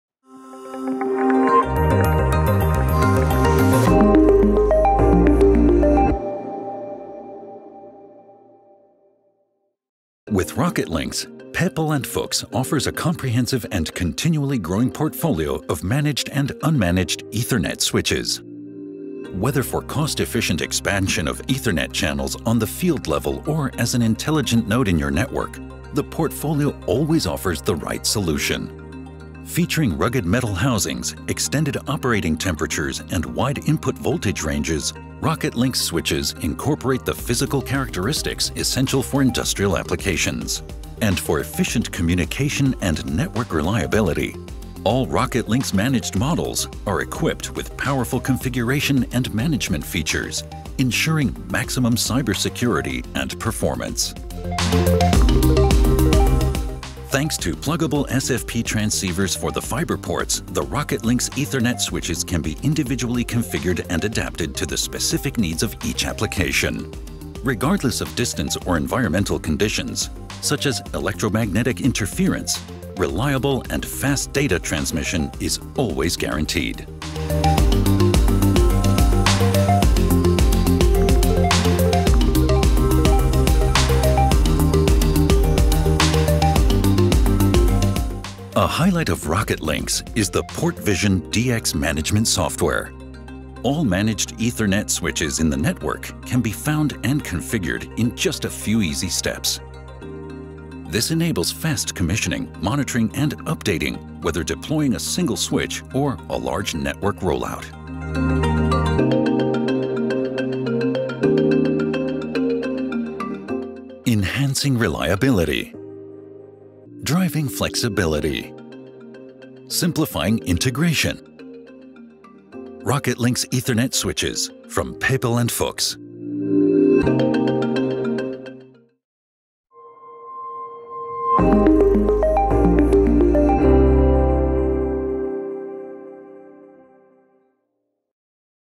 RocketLinx® Ethernet Switches: Full-Featured, Advanced Security, Advanced Network Performance
Ethernet switches provide the means for efficient and reliable communication of network data, ensuring that devices from field to enterprise level reliably communicate across the network.  Pepperl+Fuchs is introducing RocketLinx® Ethernet switches that complement its portfolio of field-to-cloud communication. Furthermore, managed Ethernet switches ensure advanced cyber security and maximum network performance.